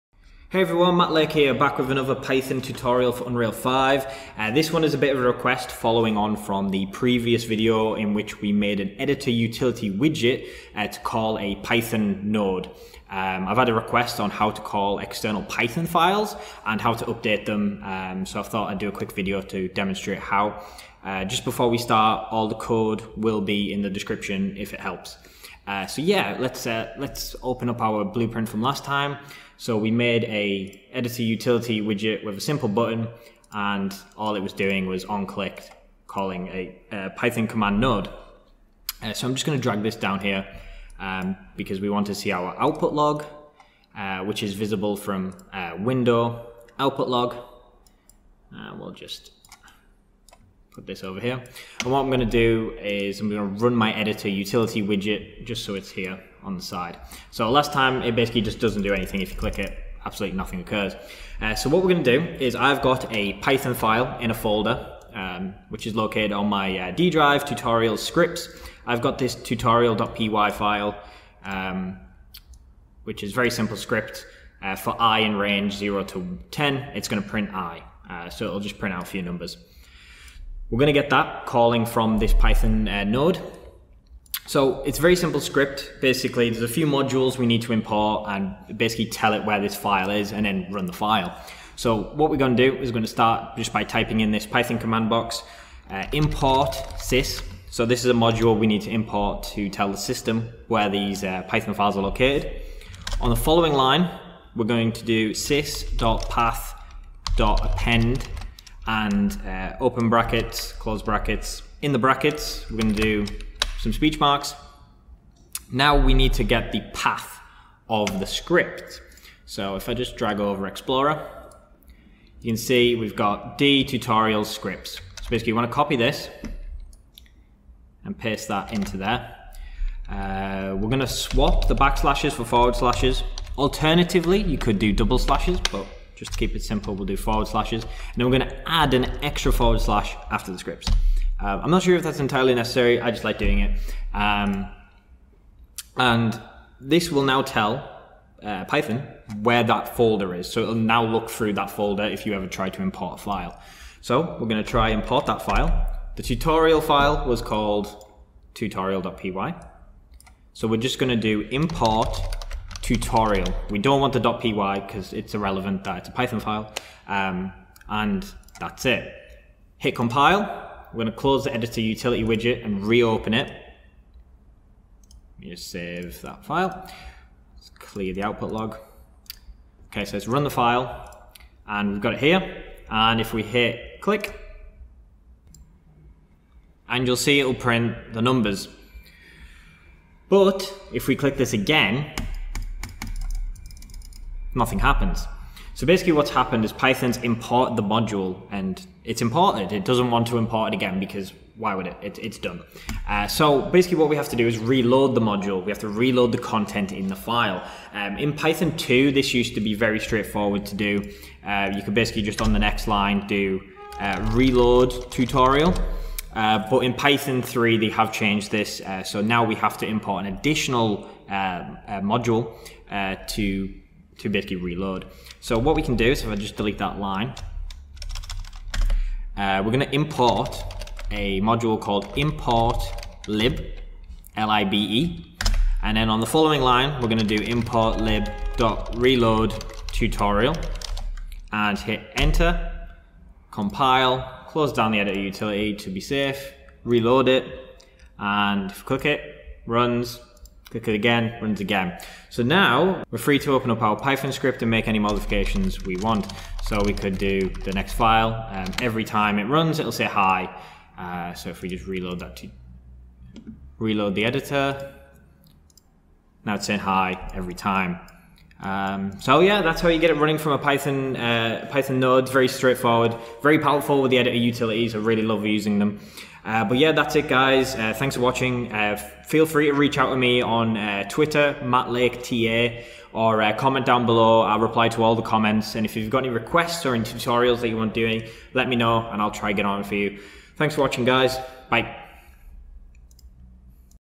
Execute Python Files In Blueprints Utilities - Unreal Engine 5 Tutorial
Unreal Python Documentation

A tutorial demonstration on how to run external Python files from within an Editor Utility Widget Blueprint within Unreal Engine 5.

-- CODE --
import sys
sys.path.append("D:\Tutorials\Scripts")
import tutorial

import importlib
importlib.reload(tutorial)